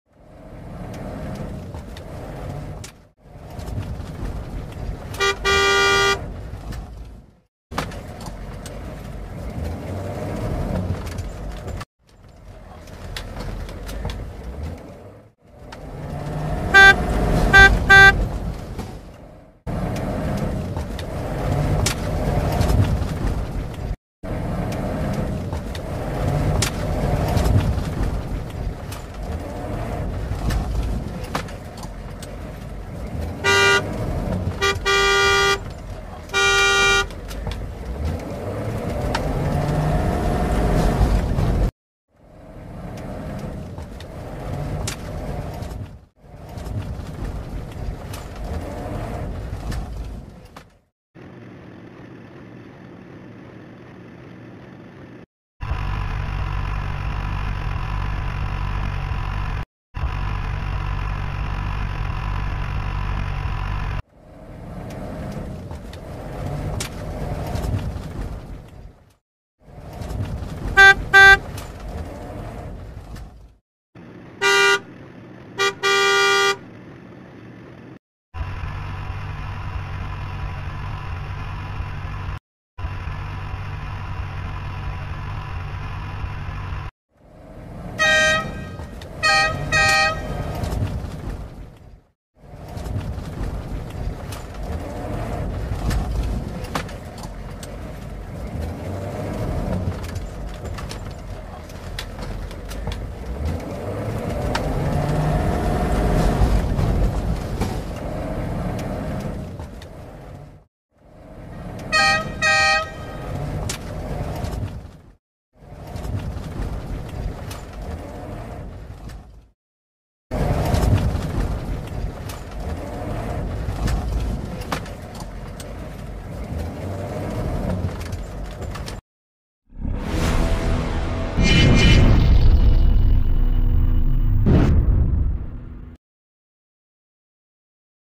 HUMMER CAR Video Green Screen. FOOTAGE ANIMATION on a green screen.
Hummer cars on a green screen for video editing. Chromakey animation of the car from different angles to insert into the video. Use it in your videos.

CHANNEL ARMY

AVIATION

EFFECT